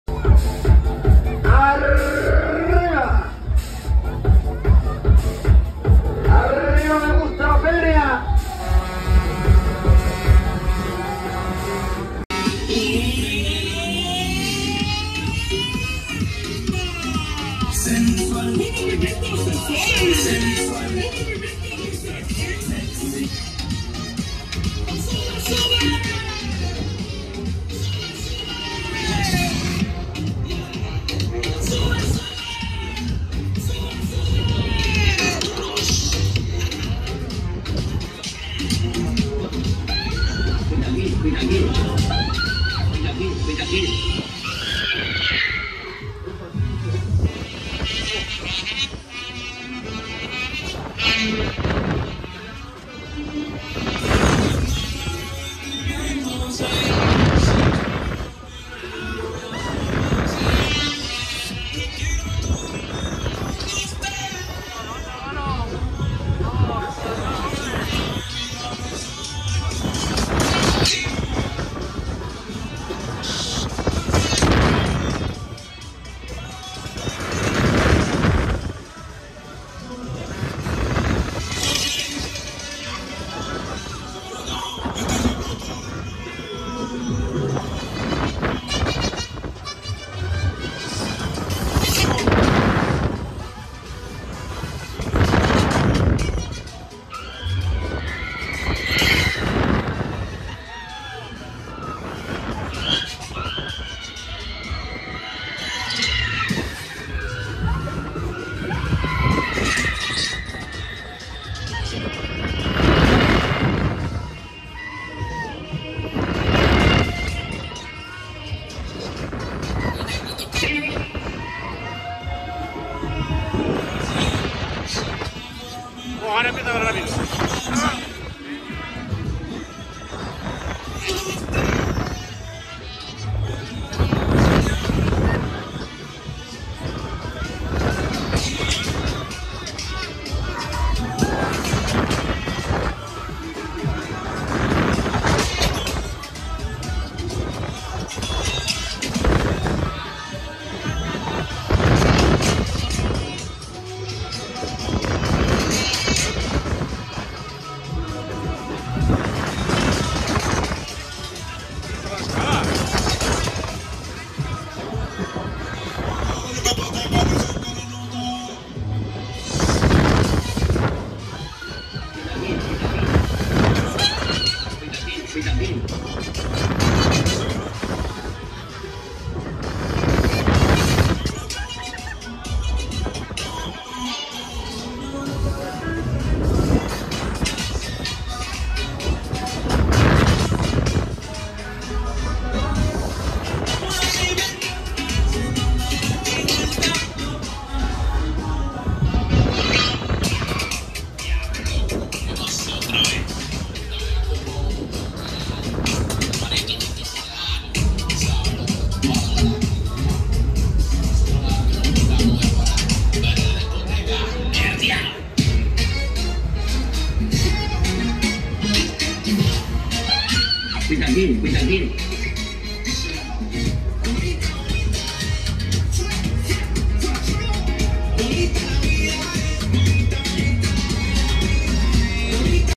Big Flight Extreme (Freak Out) OnRide en la Feria de San Mateo de Talavera de la Reina 2.024
Vídeo OnRide de la atracción Big Flight Extreme Atracciones J.J. Flores en la Feria de San Mateo de Talavera de la Reina 2.024. Vídeo realizado la página de las ferias.